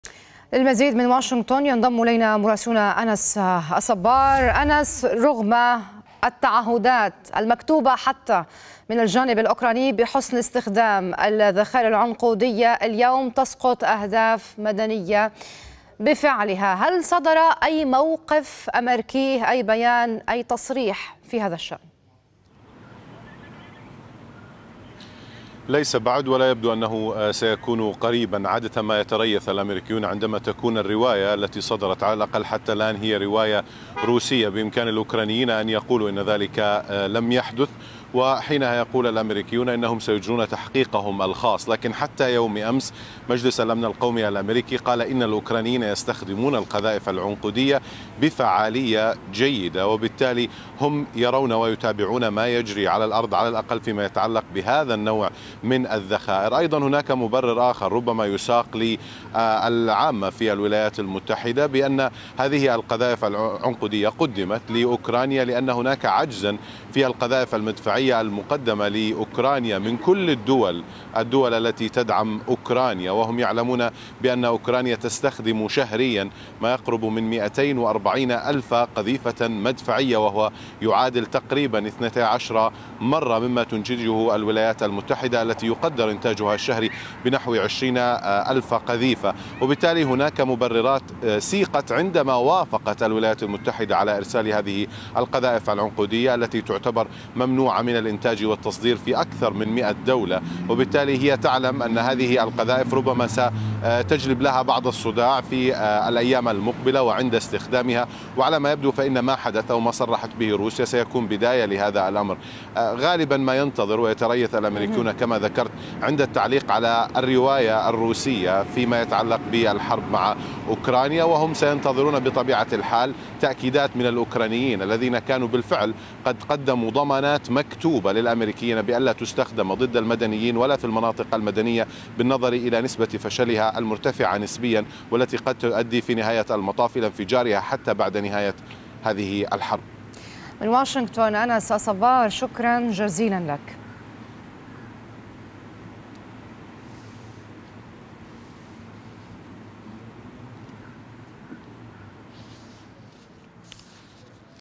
حاكم بيلغورود: كييف شنت هجوما بـ 3 قنابل عنقودية على قرية في المنطقة
روسيا تتهم أوكرانيا باستخدام ذخائر عنقودية لقصف بيلغورود، واستهدافِ صحفيين في زابورويجيا وتتوعد بالرد على هذه الهجمات.